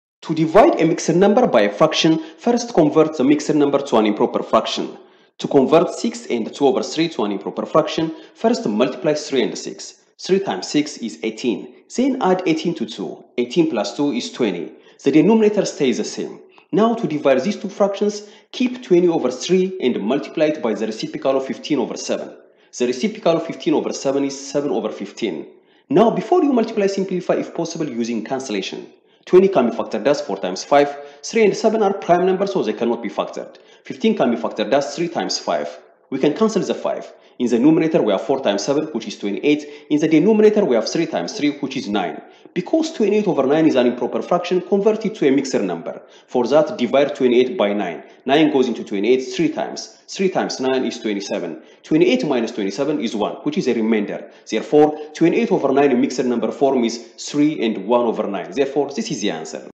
6 2/3 ÷ 15/7 = ? Divide the Mixed number by the Fraction | Prealgebra
This prealgebra math video explains how to divide  a mixed number or mixed fraction by a fraction.